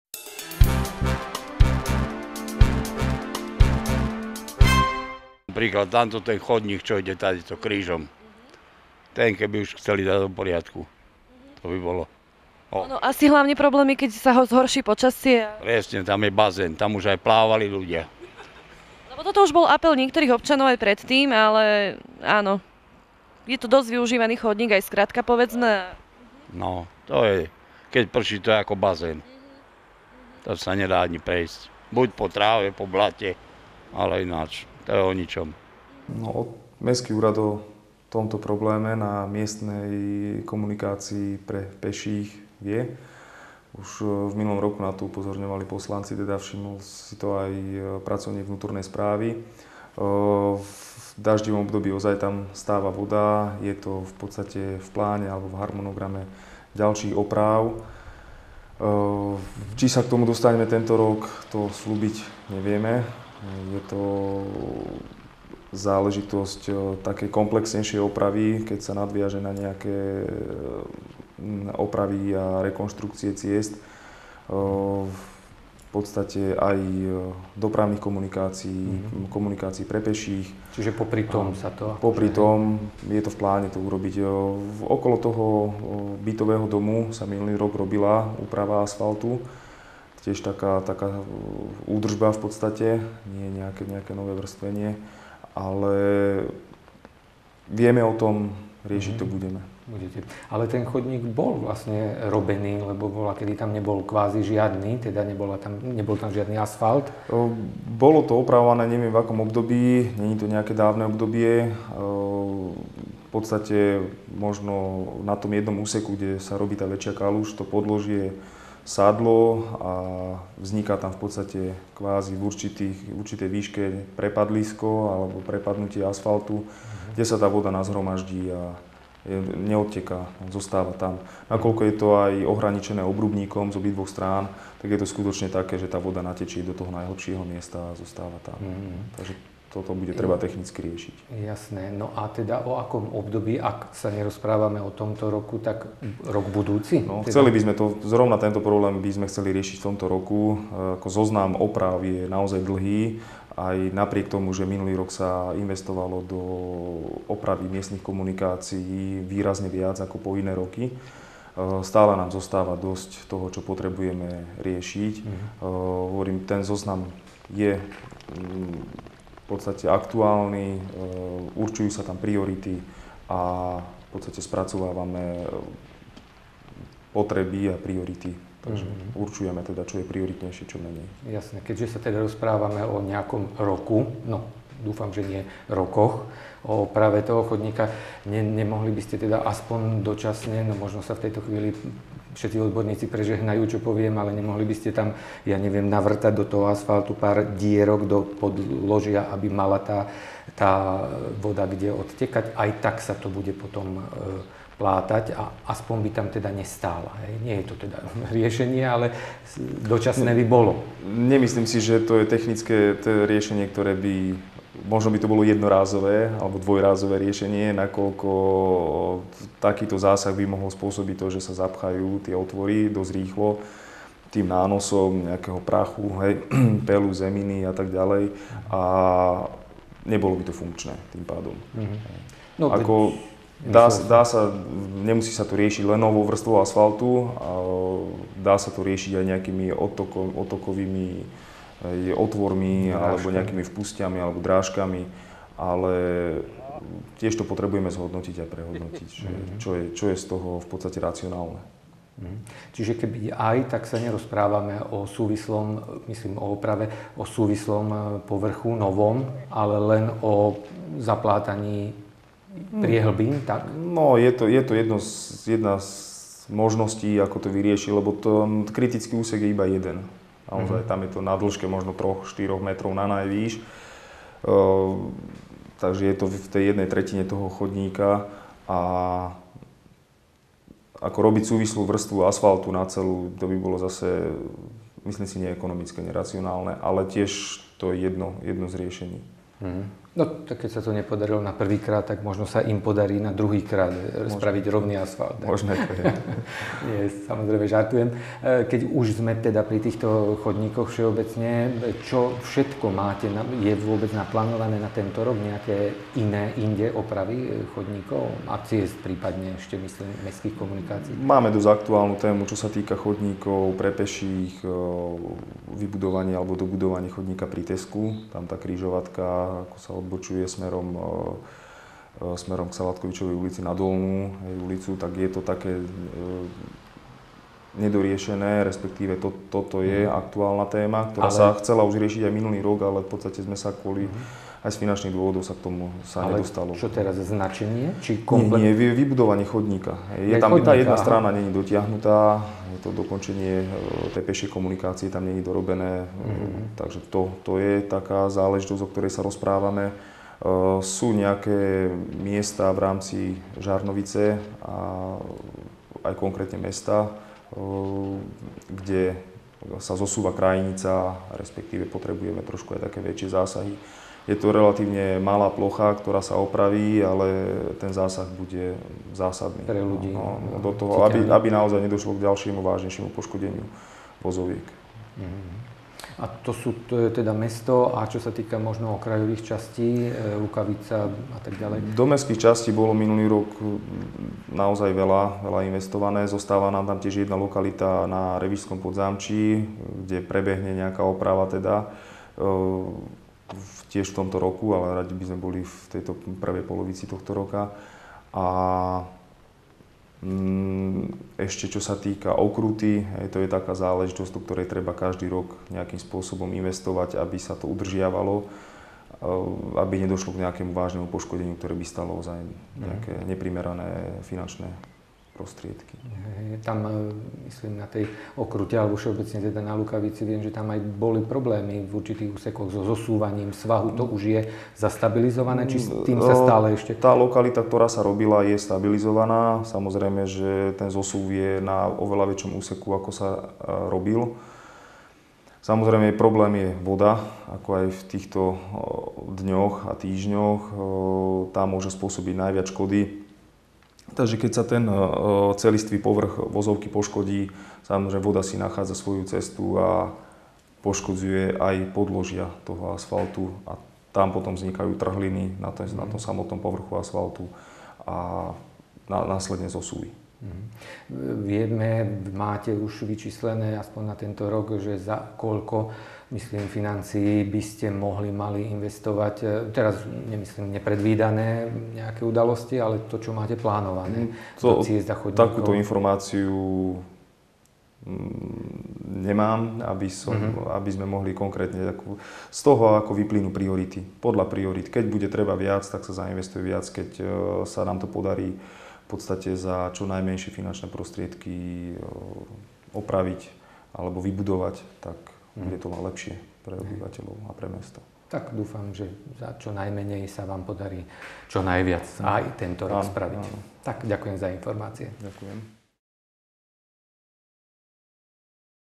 Krivý chodník
Rozhovor s vedúcim vnútornej správy v Žarnovici o zle prevedenej oprave chodníka v minulosti.